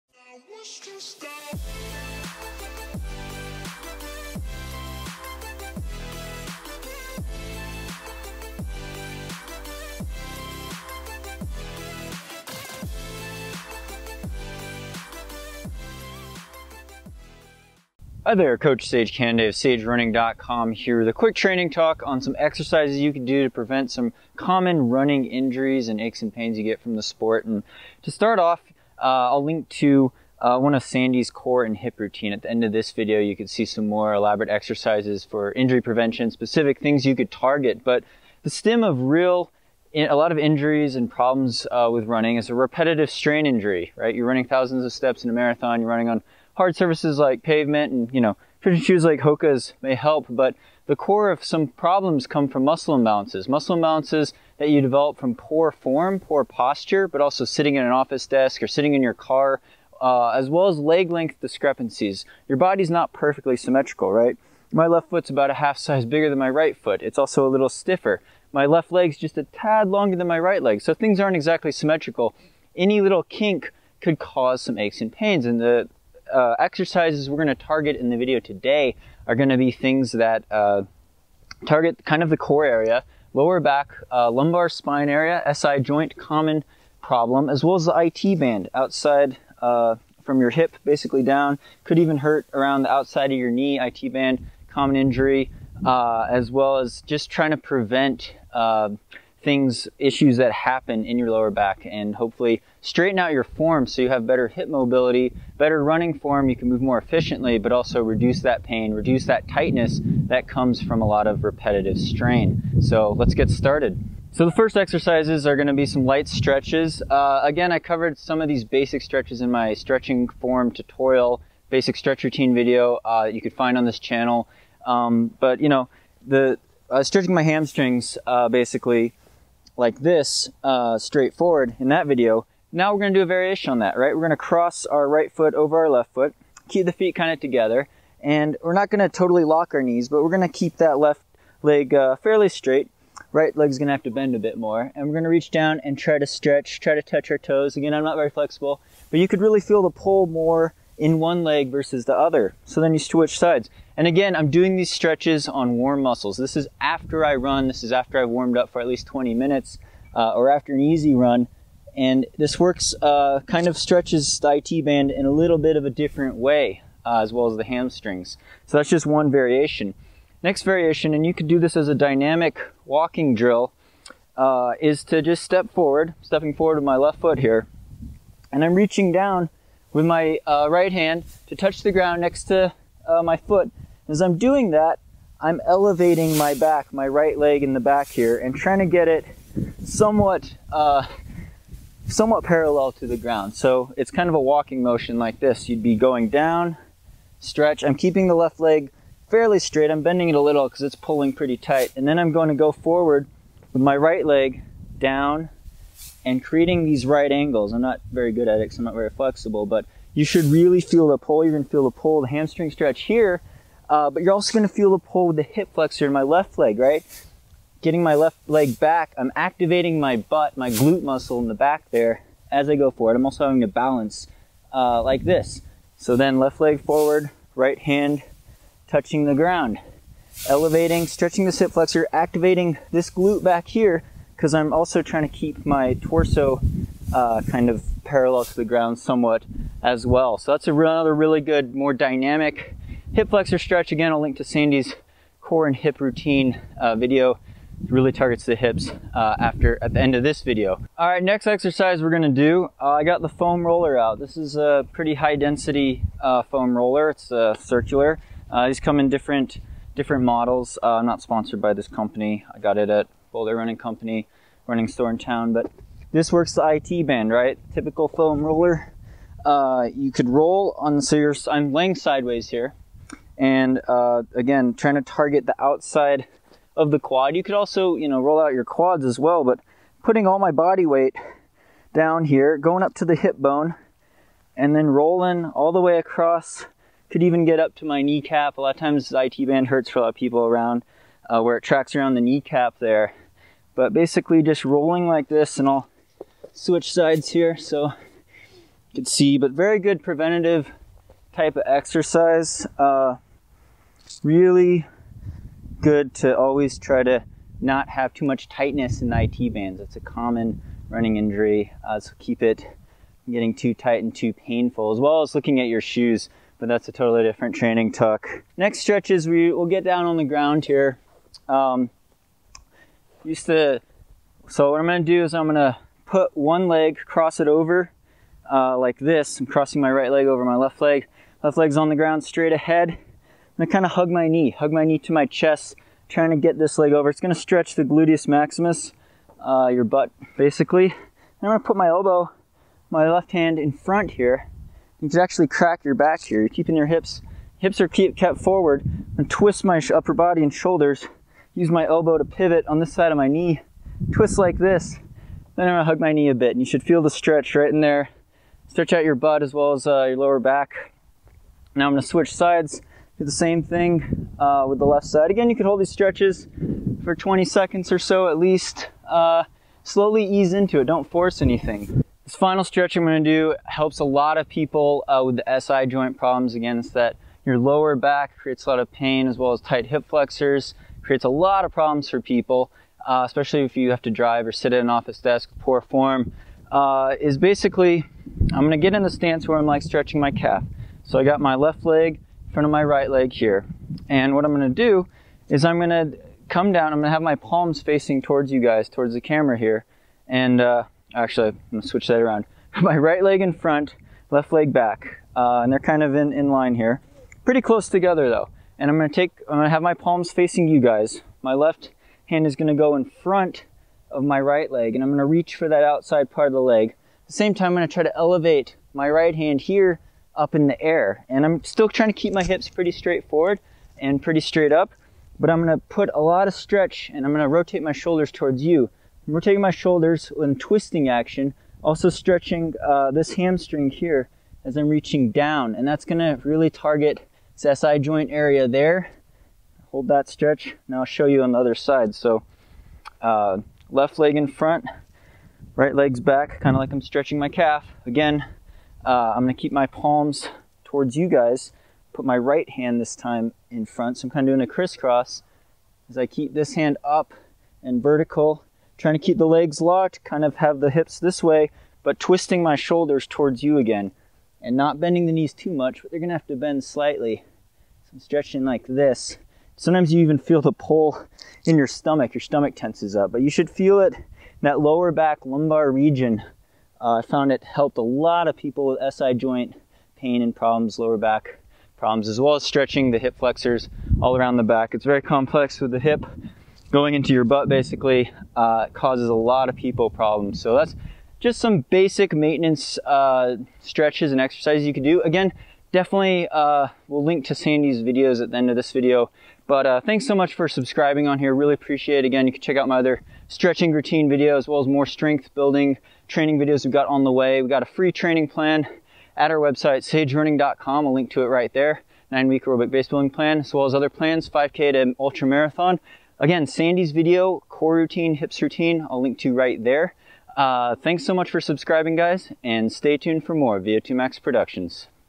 KEY EXERCISES AND STRETCHES FOR RUNNERS: REDUCE LOWER BACK/HIP/KNEE PAIN | Sage Running
Target lower back (SI joint), IT band areas ( knee/quad/hamstrings) and the hips with these simple stretches and exercises!